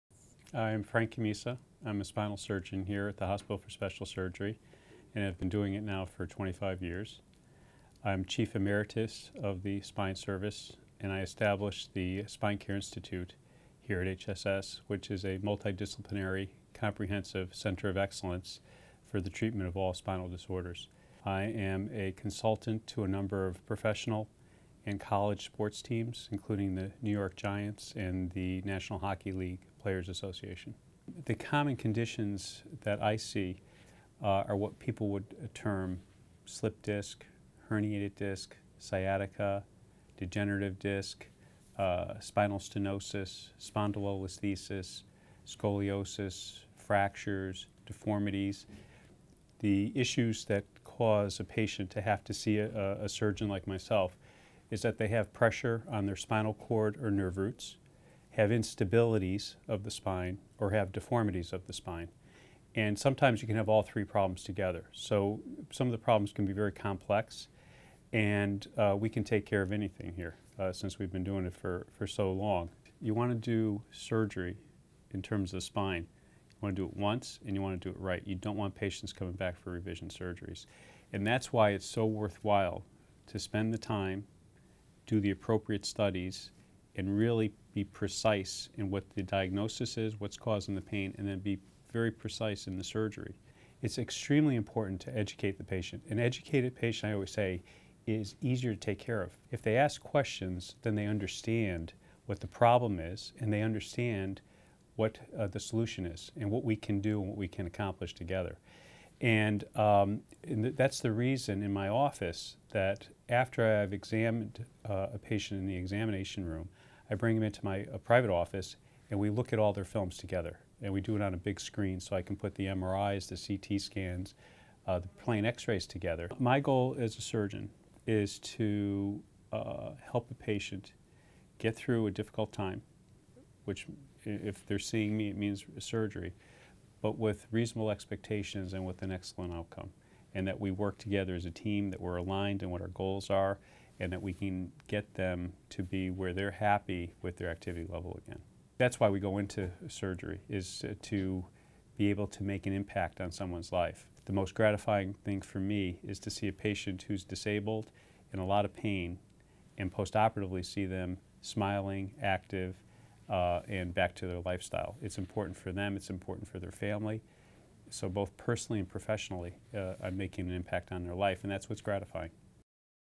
Dr. Frank Cammisa Physician Profile
Dr. Frank Cammisa, Chief Emeritus of the Spine Service at Hospital for Special Surgery, specializes in the surgical treatment of spinal disorders. His areas of expertise include minimally invasive spinal surgery, computer-assisted spinal surgery, microsurgery, athletic spinal injuries and motion-preserving procedures, including artificial disc replacement.